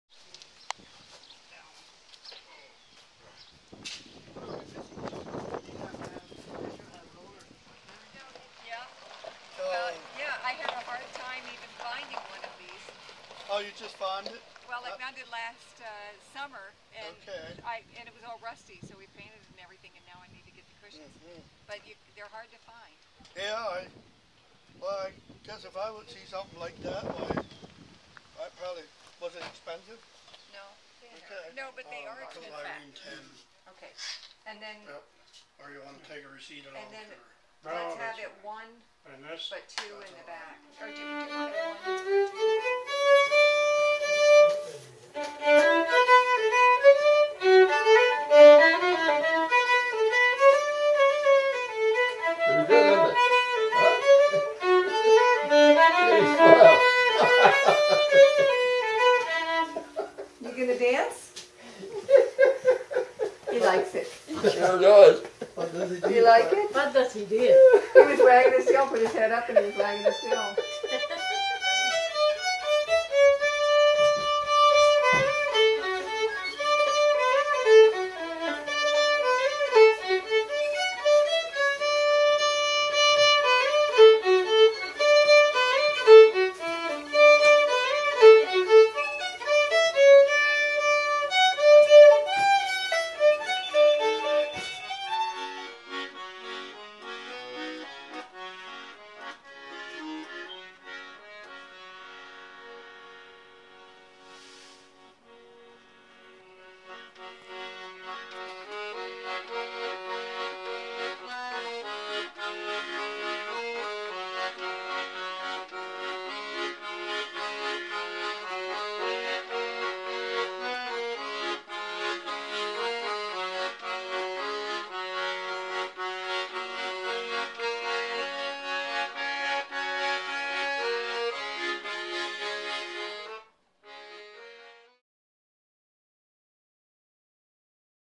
Getting glider cushions and a lesson in culture differences
A trip to Dundee, New York - and the upholstery shop of Mennonite Mahlon Burkholder - proved to be enlightened for us (about the family and their customs) and music, with a violin concert provided by Admiral Fox.